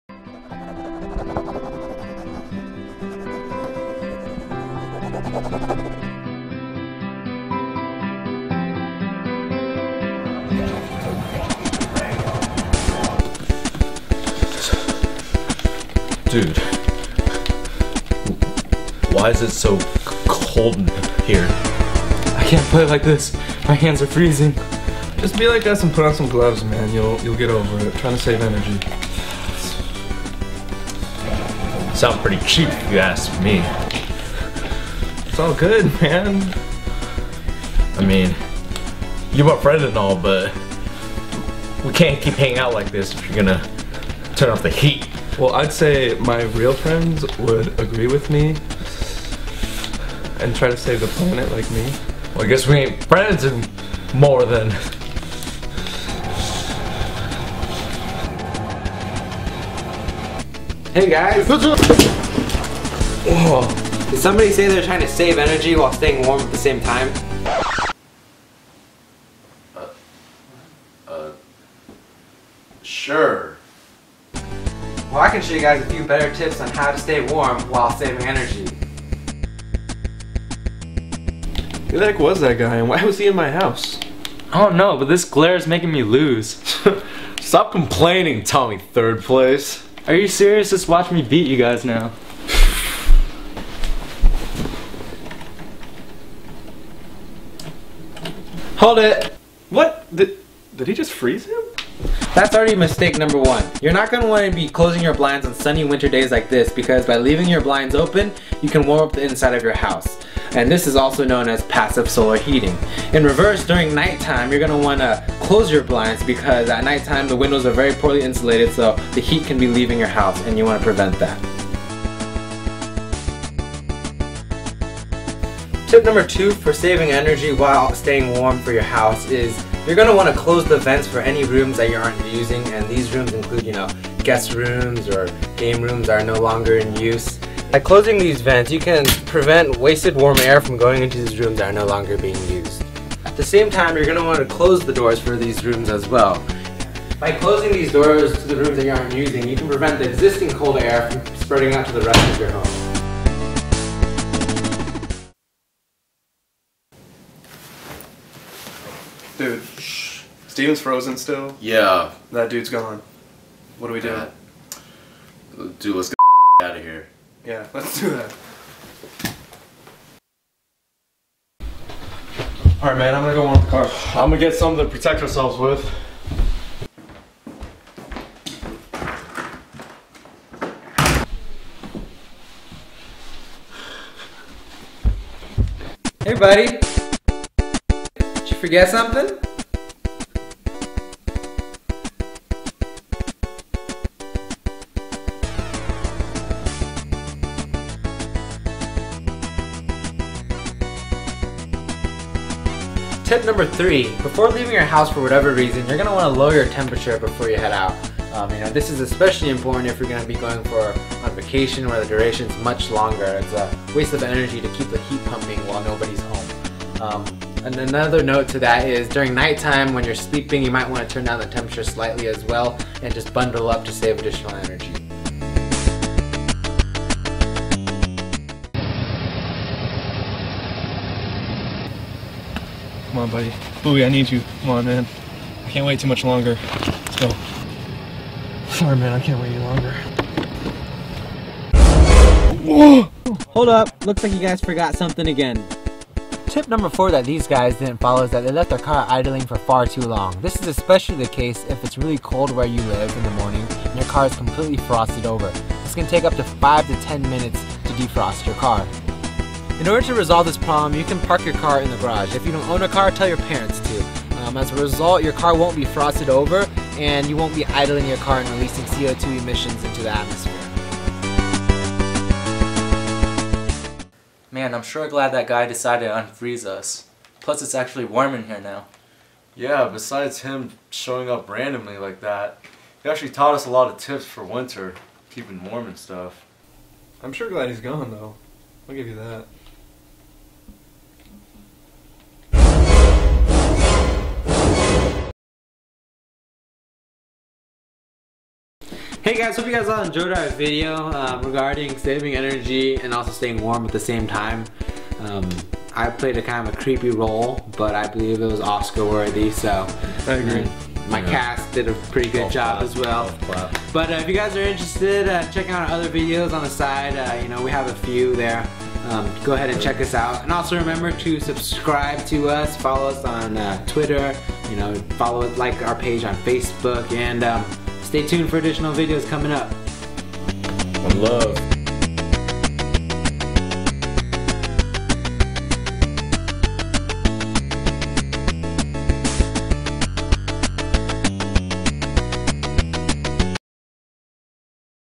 Saving Energy During Winter/Cold Weather
***closing vents update***
We're sorry to inform you that closing vents for unused rooms may not always be the best option. It is generally okay to close maybe 1 or 2 vents here and there, but closing too many vents may cause your furnace to over heat.

Hi everyone. Ever wonder how to conserve energy and stay warm when it's cold out? This is especially important during the winter time. We know it's not exactly winter anymore, but around the pacific northwest where we live, it still often snows in February and March! We hope this video provides some helpful tips for everybody.

Originally, this video was intended to be purely educational, but we decided to take a little horror spin on it. As a result, it made me (Matthew) look like a complete weirdo during this video lol. Hope you all enjoy!

Special Thanks to Sean Shkurhan for filming.